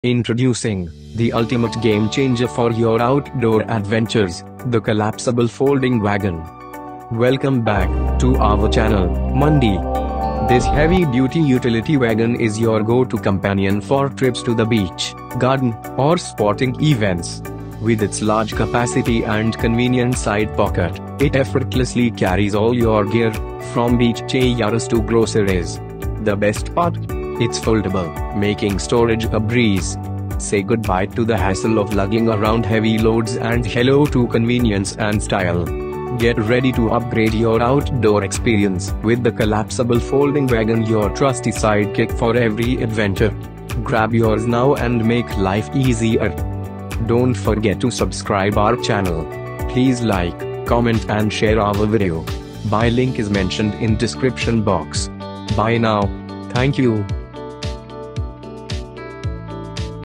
Ultimate Collapsible Wagon Review: Heavy Duty Collapsible Utility Cart, Folding Wagon for Outdoor
Today’s Deal For India

Ultimate Collapsible Wagon Review: Heavy Duty Collapsible Utility Cart, Folding Wagon for Outdoor Adventures

Introducing the ultimate game-changer for your outdoor adventures: the Collapsible Folding Wagon!

Welcome back, to our channel, Manddi

This heavy-duty utility wagon is your go-to companion for trips to the beach, garden, or sporting events. With its large capacity and convenient side pocket, it effortlessly carries all your gear, from beach chairs to groceries. The best part? It's foldable, making storage a breeze. Say goodbye to the hassle of lugging around heavy loads and hello to convenience and style. Get ready to upgrade your outdoor experience with the Collapsible Folding Wagon – your trusty sidekick for every adventure! Grab yours now and make life easier!

Buy Link is Mentioned in Description Box